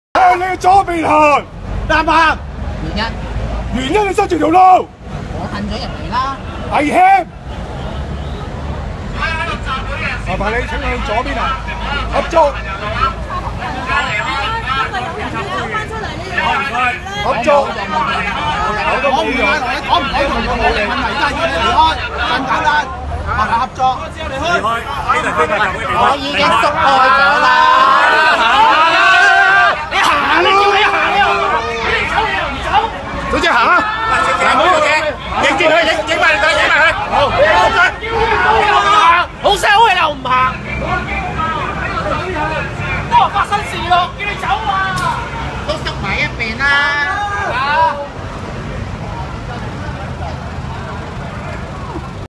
unknown
【雨傘革命，佔領街道】

旺角實況：今晚較早時間攝於山東街，警員以不禮貌態度及動作，強行要求一男子離開。

（片段由讀者提供）
支持警察，對佔中暴民，是正確處理。

係要死去高危地區，叫你走又唔走，一陣又話俾人打，咁只能怪自己犯賤囉！怨不了誰！

個大標題就吾啱啦，個肥仔多事警察由有禮叫佢行到發火，前面大條路比佢行分明搞事之後又話警察無禮貌……多如﹏

你依家去名店購物定係入住六星級酒店呀！要點先係有禮貌？已經叫咗麻煩行開唔好阻住馬路啦！on99

其實真係叫佢走佢唔走咋喎